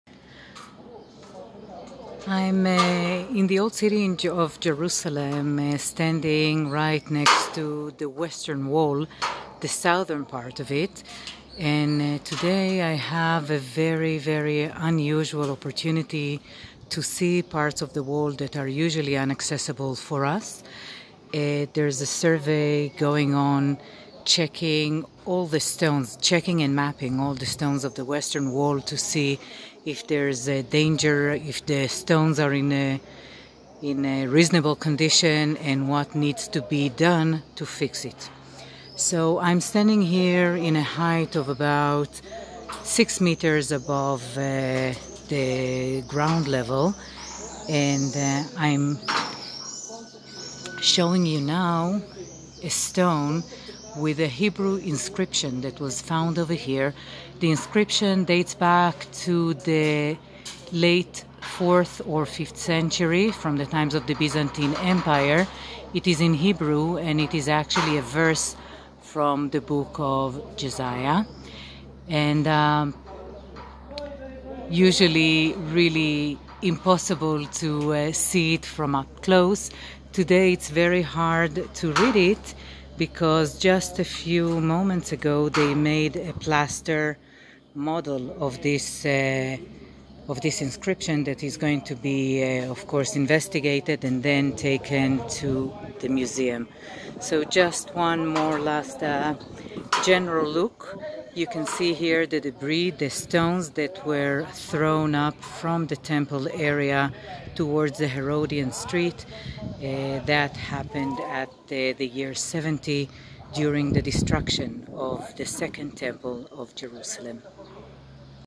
Rare Video: The Western Wall (Wailing Wall), Jerusalem from an angle you haven't seen in your life
Davidson Center, the Jerusalem Archaeological Park, is located nearby the Western Wall in the Old City. Davidson Center is one of the Jerusalem’s top tourist attractions due to its historical and archeological significance and, of course, its beauty. You will find archaeological finds and displays from the First& Second Temple periods: the ancient city wall, the Temple’s staircase, a preserved ancient street, ritual immersion baths, and stores